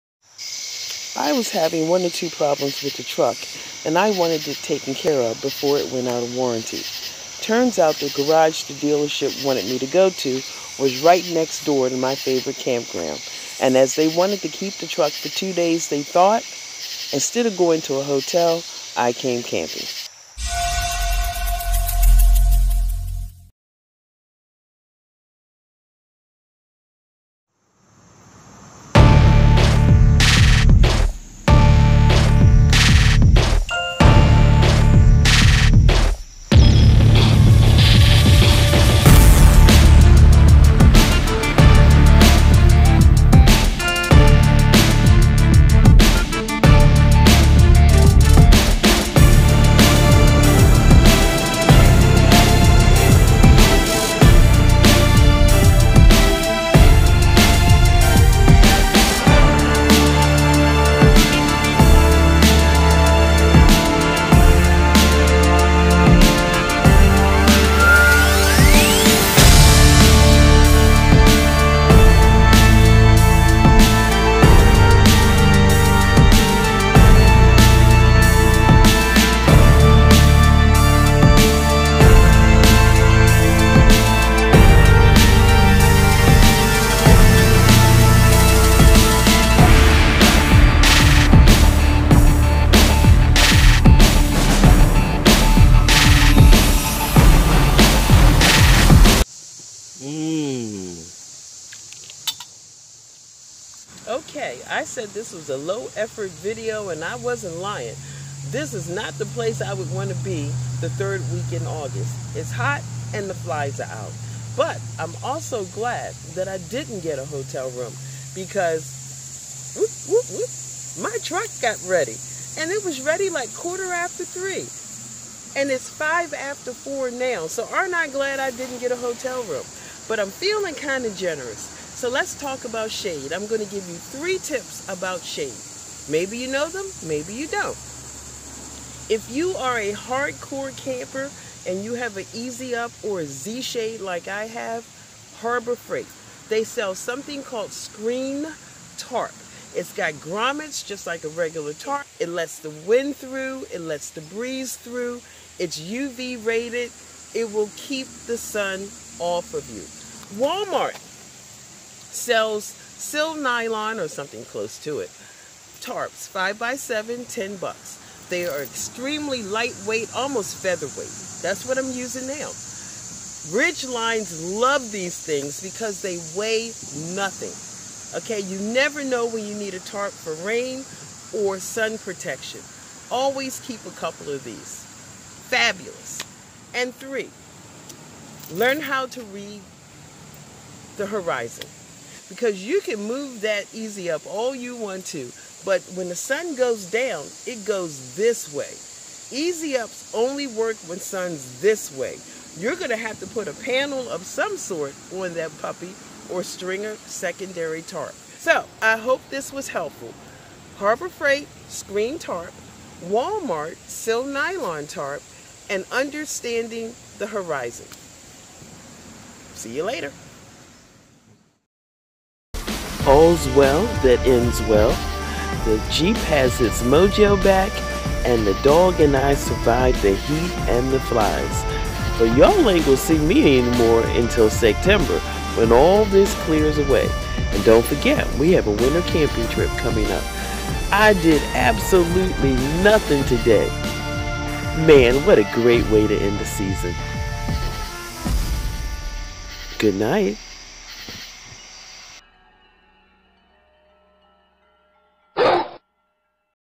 HEAT + FLIES = BEST CAMPING TRIP EVER!!!
The Heat and the Flies gave me a reason to .film lazy...all talk and showing you stuff but not really doing anything : ) OMG does that make this video a .V-LOG?? ..lol